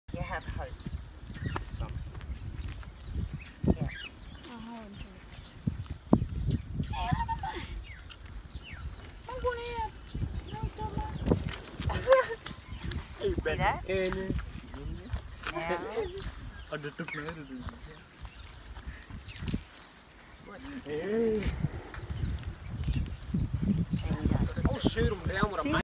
Mungalla Station North Queensland
I was very honored to be invited to spend the day with amazing young people of the Mungalla Station Aboriginal Community